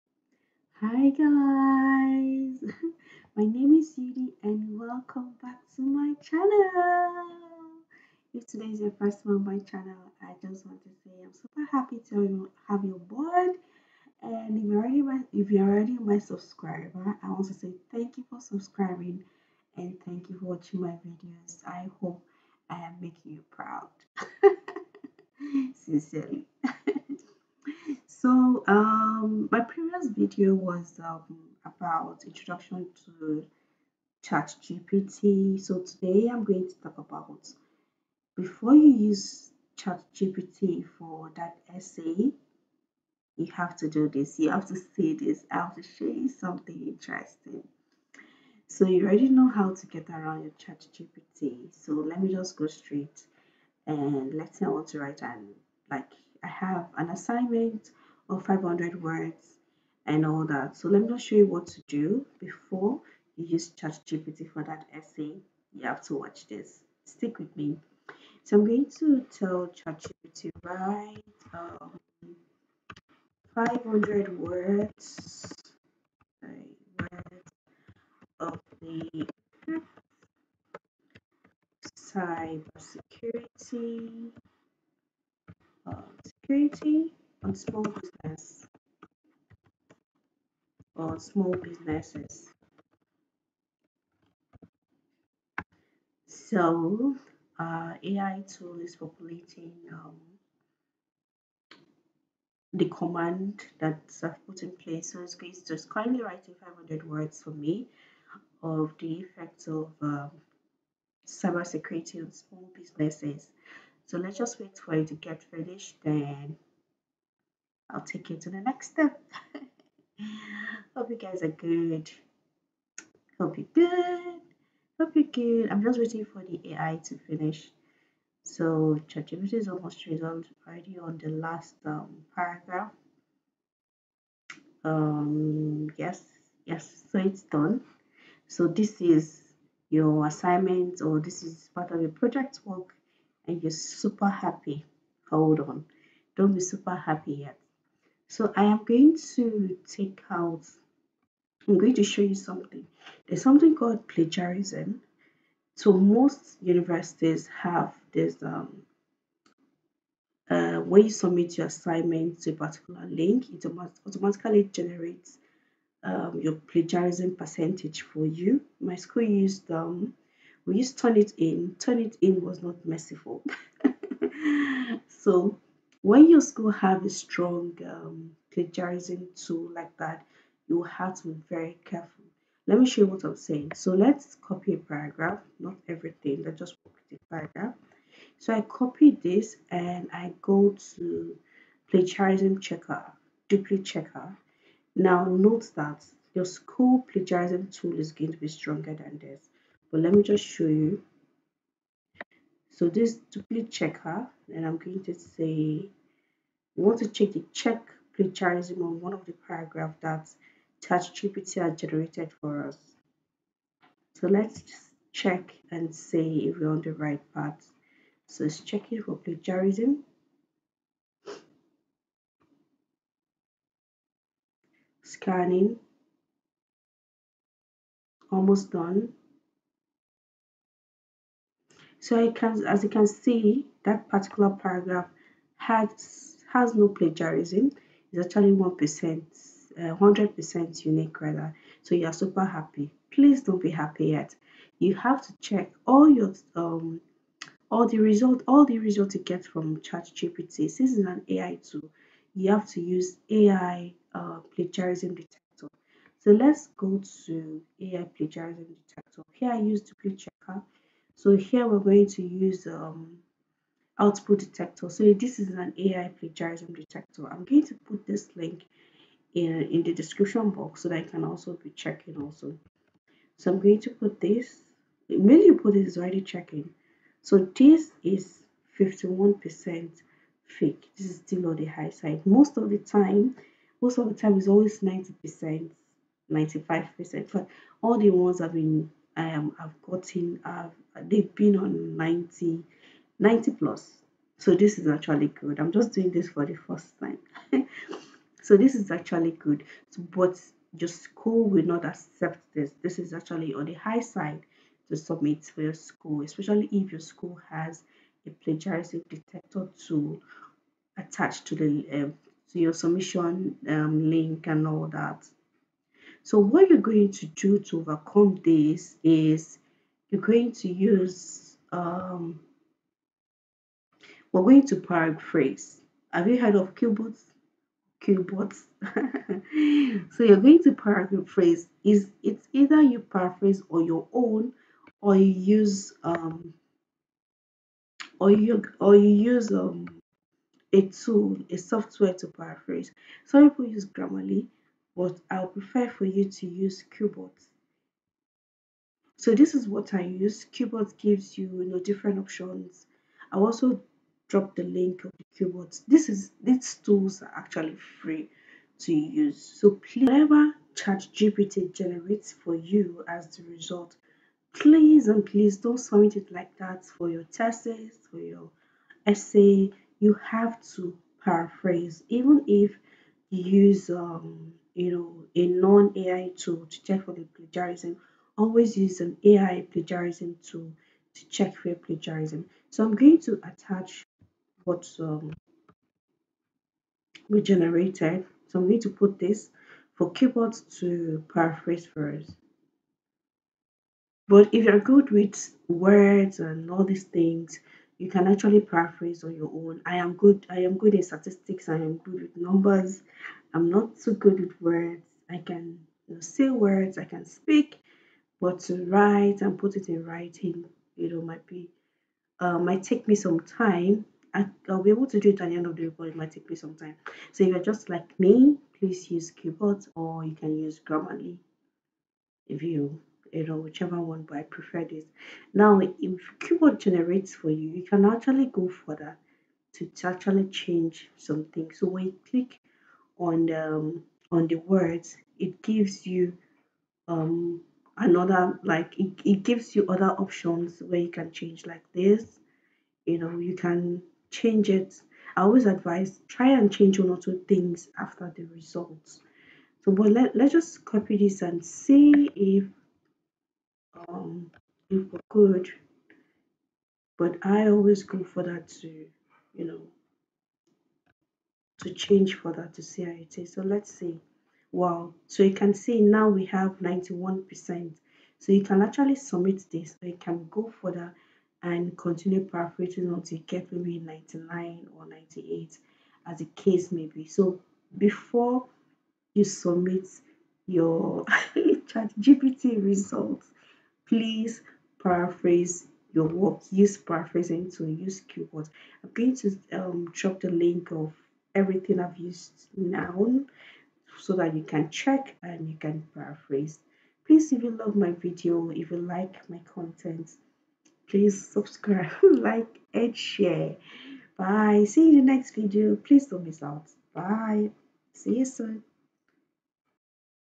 HOW TO GET PAST CHATGPT PLAGIARISM | Perfect ChatGPT Essay without Plagiarism
ChatGPT Essays comes with high plagiarism, today's tutorial shows you how to bypass that and have perfect Essay, Thesis, etc